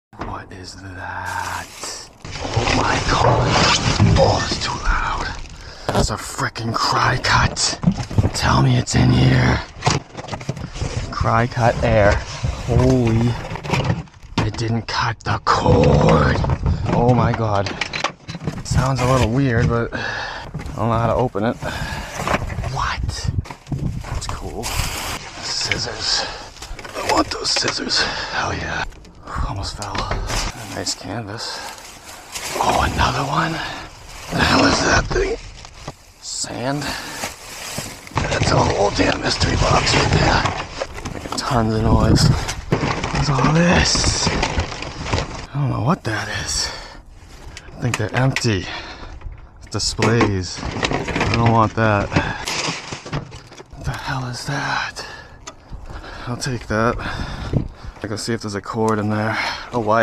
Made over 400$ in a dumpster
This dumpster was packed with brand new goods !!